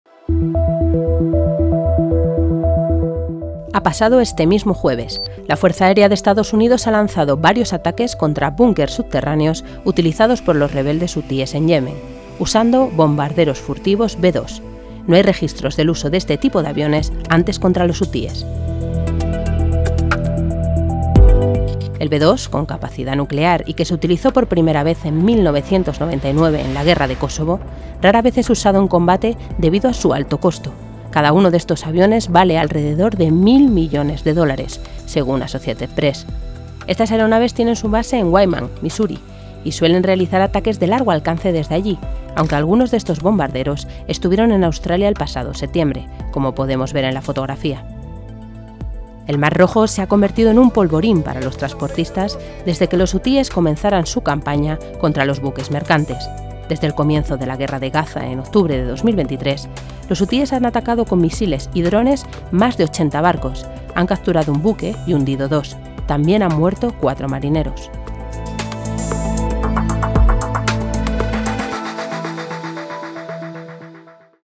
B-2 Spirit, el bombardero furtivo de 1.000 millones que está usando Estados Unidos en Yemen
RAQUEL MORENO. Ha pasado este mismo jueves. La Fuerza Aérea de Estados Unidos ha lanzado varios ataques contra búnkeres subterráneos utilizados por los rebeldes hutíes en Yemen. No hay registros del uso de este tipo de bombardero antes contra los hutíes.

El B-2, con capacidad nuclear y que se utilizó por primera vez en 1999 en la guerra de Kosovo, rara vez es usado en combate debido a su alto costo: cada uno de estos aviones vale alrededor de 1.000 millones de dólares, según Associated Press.

Estas aeronaves tienen su base en Whiteman, Misuri, y suelen realizar ataques de largo alcance desde allí, aunque algunos de estos bombarderos estuvieron en Australia el pasado septiembre, como podemos ver en la fotografía.

El Mar Rojo, se ha convertido en un polvorín para los transportistas desde que los hutíes comenzaron su campaña contra los buques mercantes. Desde el comienzo de la guerra de Gaza en octubre de 2023, los hutíes han atacado con misiles y drones más de 80 barcos, han capturado un buque y hundido dos. También han muerto cuatro marineros.